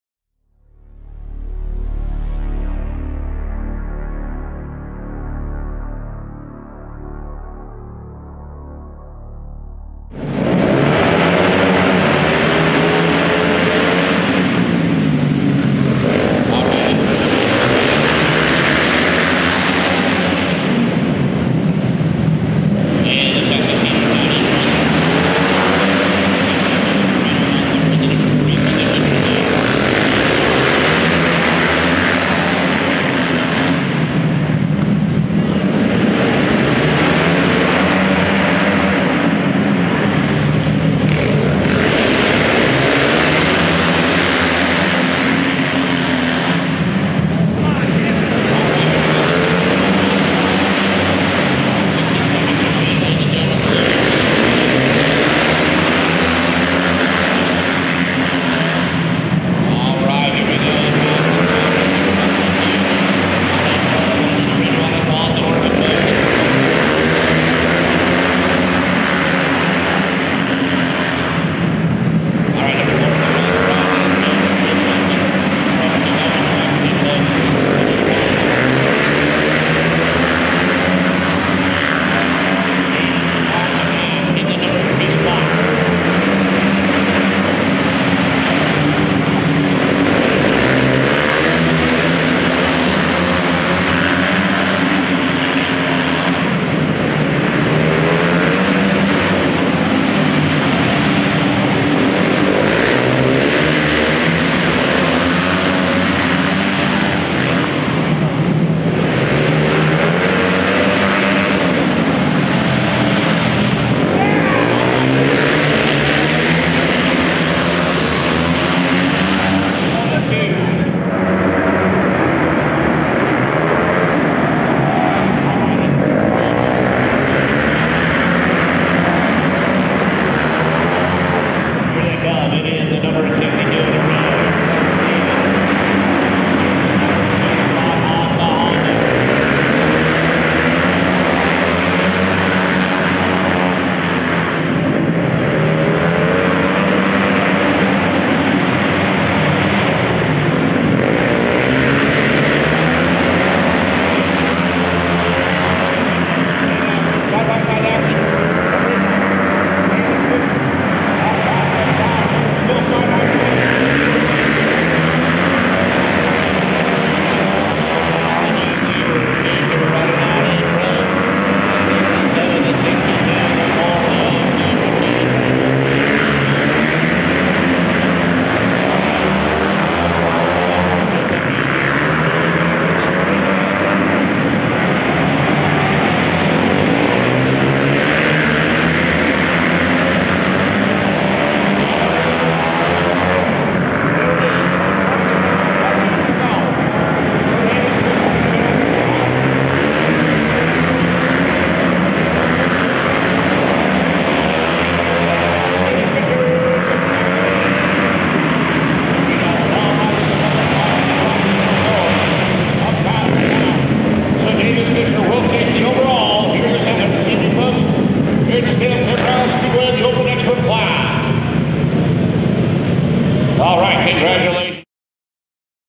Davis Fisher and Andy DiBrino Open Expert Main Salem Speedway 1-10-14
T. J. Burnett Farms sponsored rider Dashin' Davis Fisher and Road Racer Andy DiBrino with Jacob Lehmann, Austin Helmholz, Travis Ohge, and new comers to the class this year Hale Bachmann and Aidan Brown at the Salem Indoor Winter Flat Track Series.  Was Davis was holding back at the starts so he could work his way up?